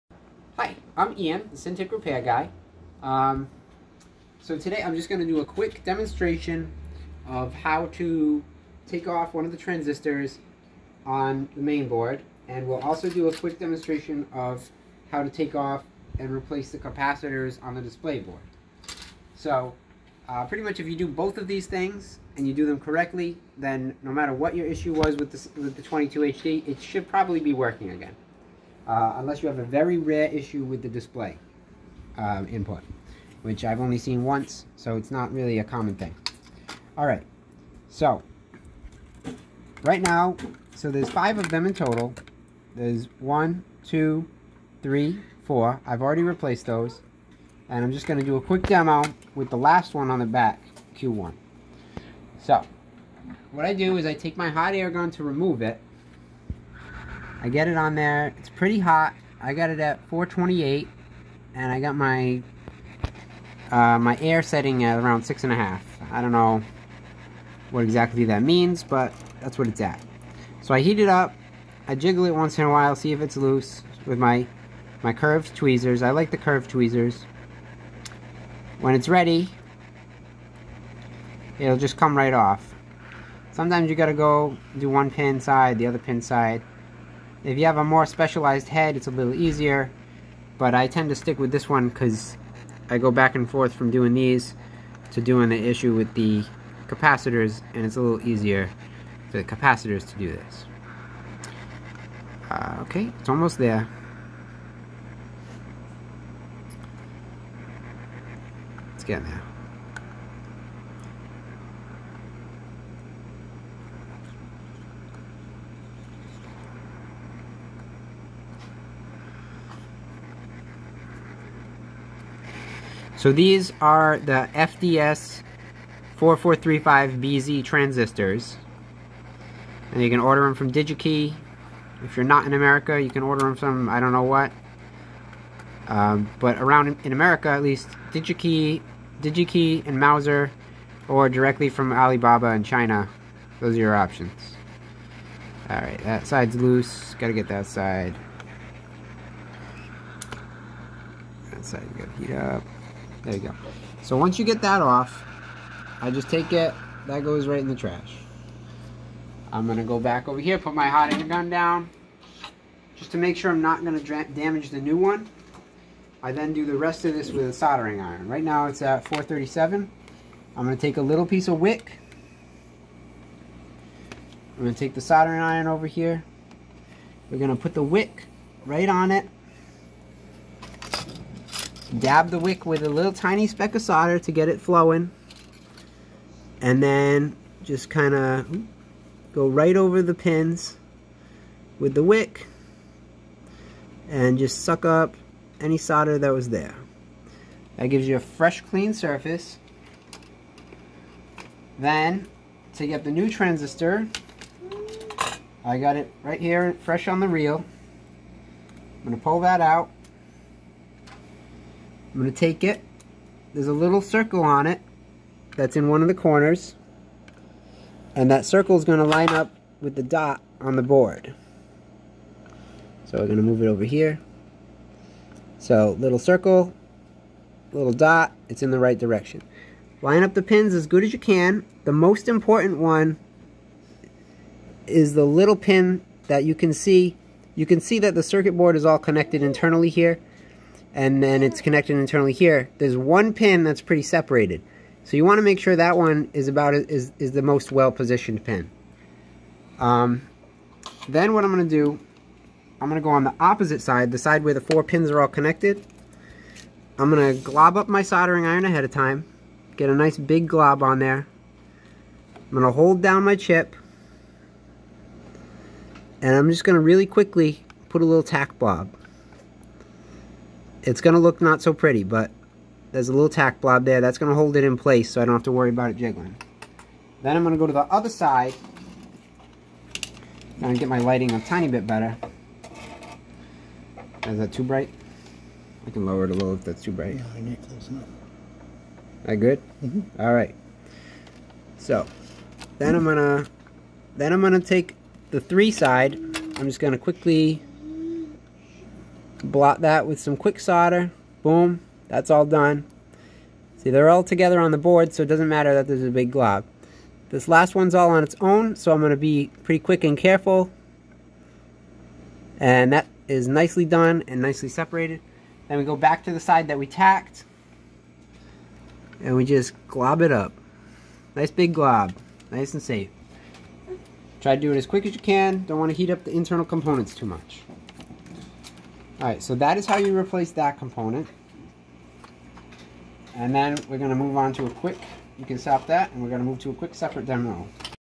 DEMONSTRATION Cintiq 22HD Transistor replacement
If you want to have your cintiq repaired or buy a refurbished Cintiq just email me at to ask what I have available, I'm working on a website, but it's a slow process.

THE PART: FDS 4435BZ

I provide this service to the world at no charge (though I definitely appreciate the tips people send me sometimes!!)